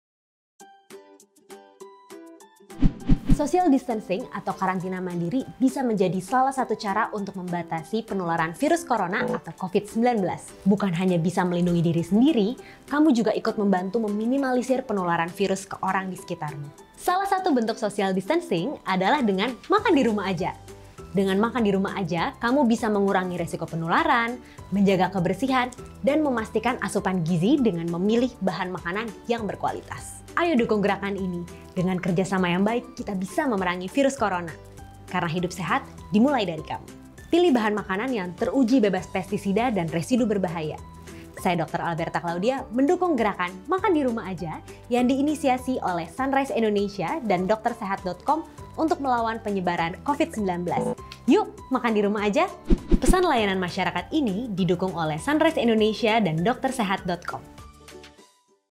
Makan Di Rumah Aja! Gerakan Mencegah Penyebaran Virus Corona (COVID-19) | DokterSehat
Doktersehat.com bersama Sunrise Indonesia mengajak kita semua untuk makandirumahaja guna mencegah penyebaran virus corona (COVID-19). Gerakan makandirumahaja adalah salah satu bentuk social distancing atau karantina mandiri yang bisa melindungi diri sendiri dan orang di sekitarmu di saat pandemi virus corona.
Dengan mendukung gerakan makandirumahaja yang diinisiasi oleh Sunrise Indonesia dan DokterSehat, kamu telah membantu pemerintah untuk melawan COVID - 19. Selain itu kamu juga bisa berkreasi untuk mengolah dan mengkonsumsi makanan sehat untuk meningkatkan daya tahan tubuh kamu.
Pembawa acara: dr. Alberta Claudia